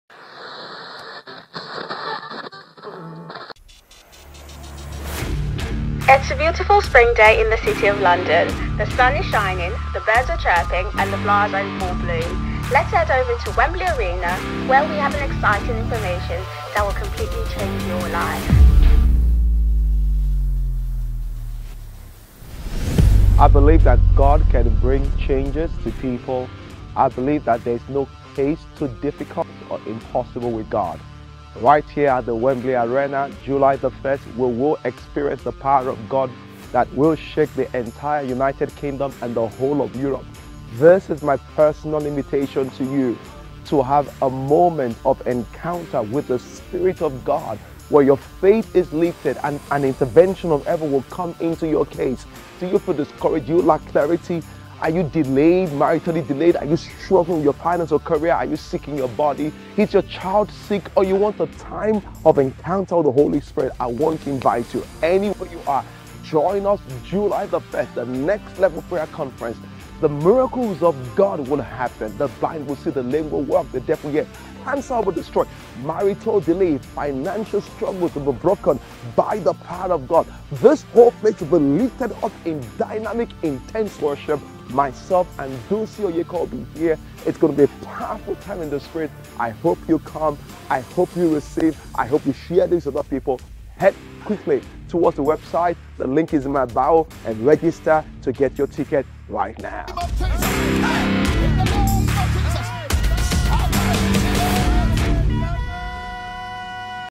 Special Invitation to the Next Level Prayer Conference
Where will you be on July 1, 2023? Plan to attend to the Next Level Prayer Conference with Pastor Bolaji Idowu and Dunsin Oyekan at Wembley OVO Arena, London